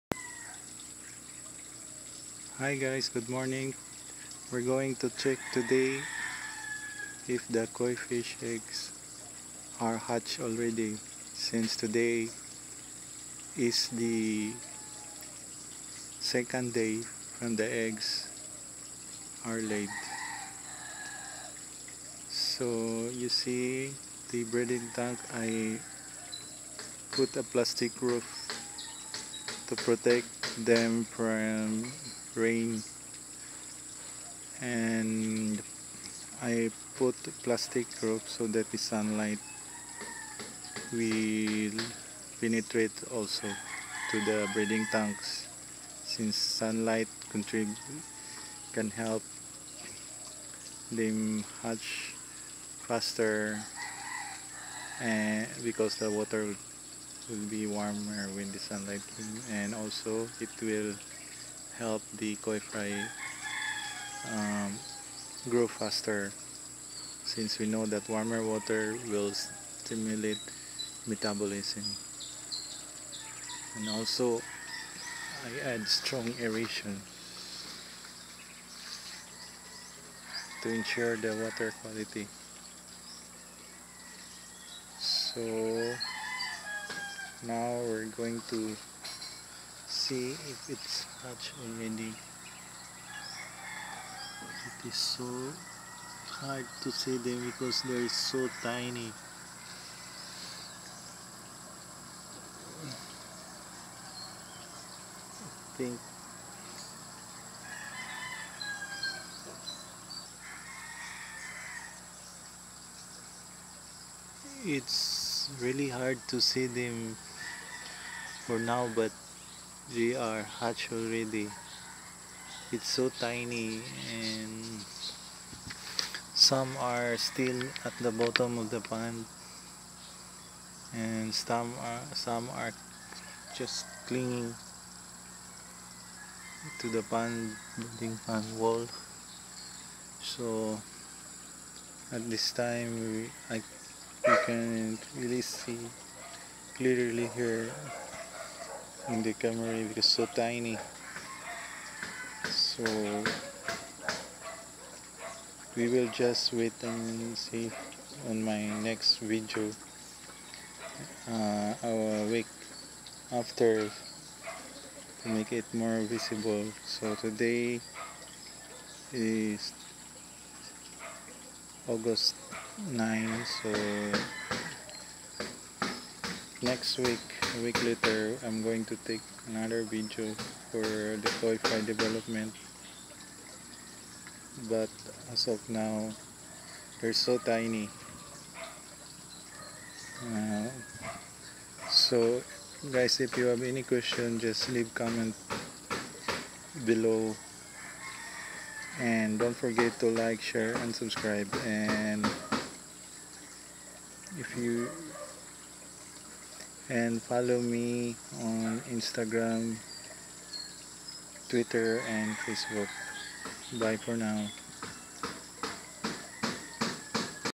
newly hatched koi fry (koi fry growth rate)
newly hatched koi fry (koi fry growth rate) This is a day old newly hatched koi fry

You can't barely see the koi fry yet. At this stage they are still clinging at pond walls and nesting media.

Their digestive system is not fully developed yet and they don't eat yet.

Also their respiration system is not fully developed also so make sure you add strong aeration.

If you want to learn more about koi breeding